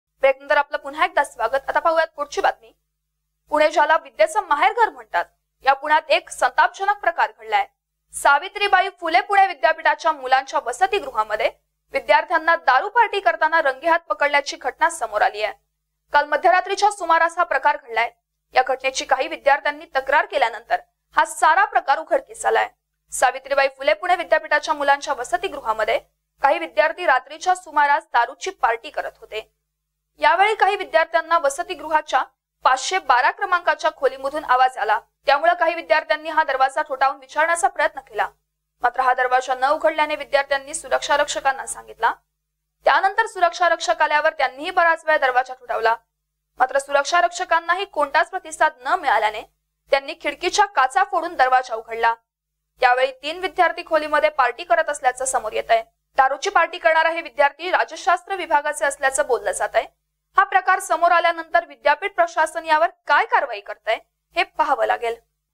PuneUniversity - विद्यापीठाच्या वसतीगृहातील संतापजनक प्रकार|MKNMARATHI
पुणे  ज्याला विद्येचं माहेरघर म्हणतात या पुण्यात एक संतापजनक प्रकार घडलाय. सावित्रीबाई फुले पुणे विद्यापीठाच्या मुलांच्या वसतीगृहामध्ये विद्यार्थ्यांना दारु पार्टी करताना रंगेहाथ पकडल्याची घटना समोर आलीये. काल मध्यरात्रीच्या सुमारास हा प्रकार घडला. या घटनेची काही विद्यार्थ्यांनी तक्रार केल्यानंतर हा सारा प्रकार उघडकीस आलाय.